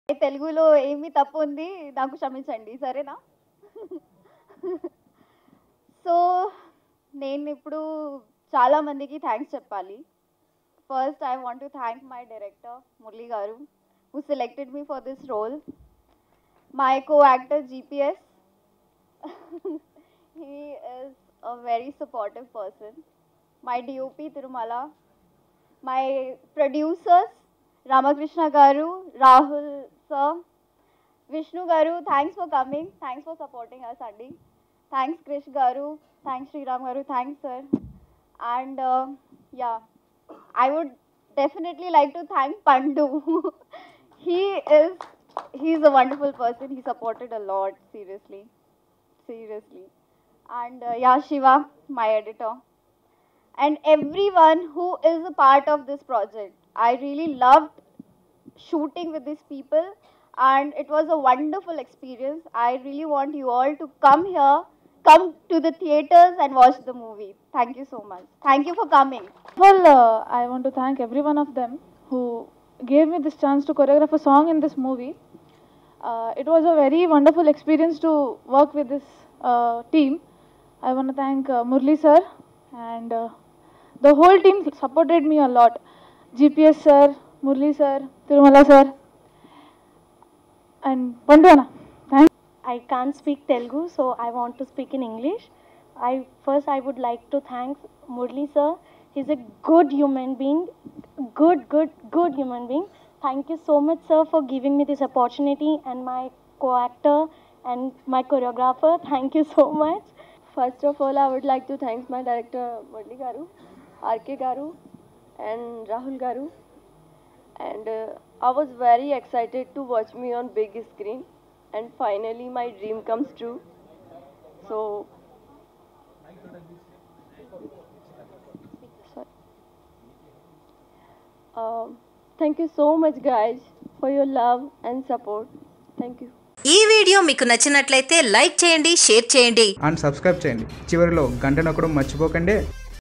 Kapilakshi Malhotra And Her Friend Very Cute And Hot Speech At Prema Pipasi Teaser Launch || LATV
Watch here Kapilakshi Malhotra And Her Friend Very Cute And Hot Speech At Prema Pipasi Teaser Launch